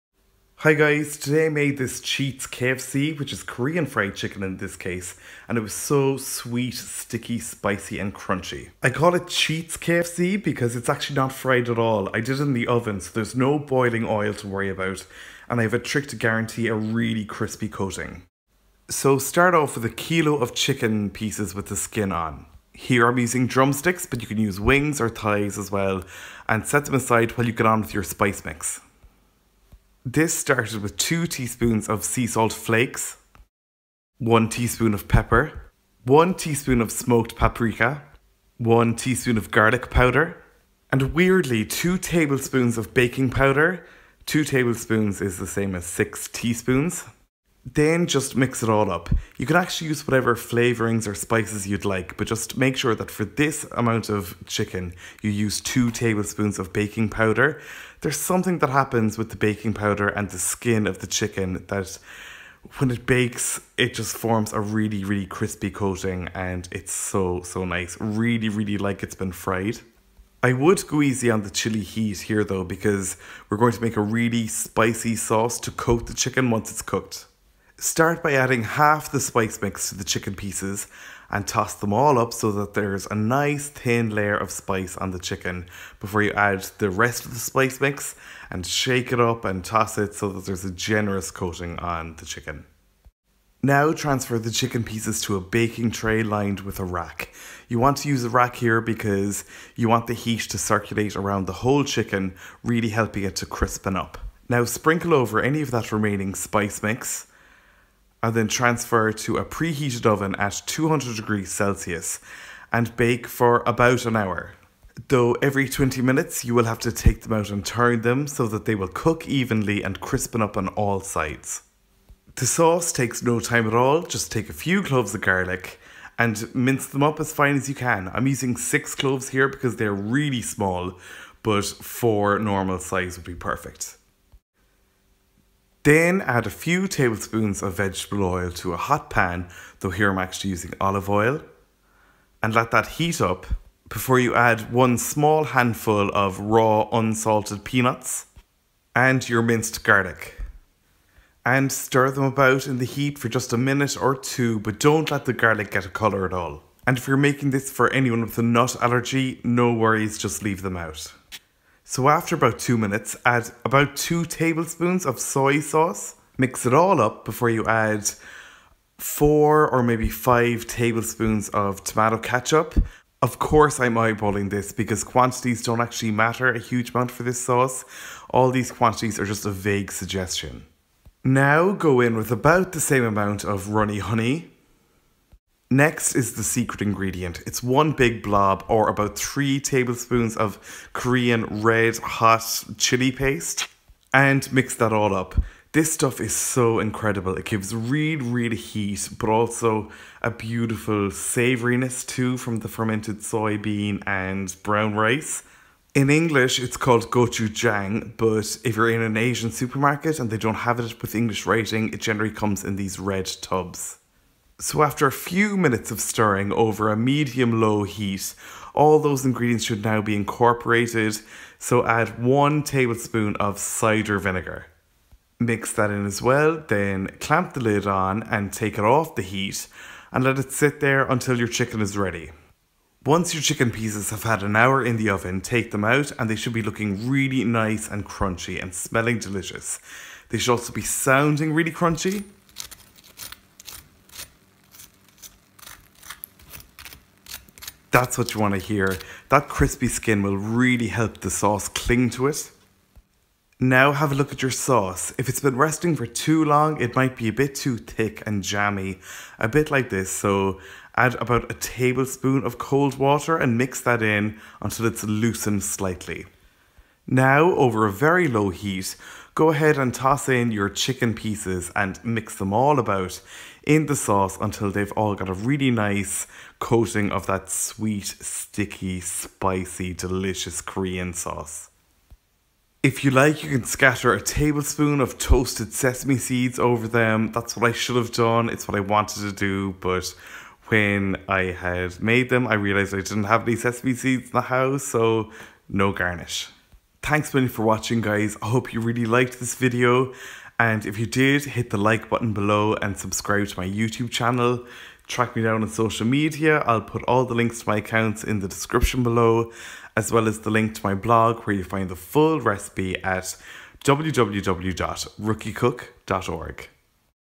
CHEAT'S KFC (Korean Fried Chicken)
Spicy, crispy and sticky Korean fried chicken, but done in the oven.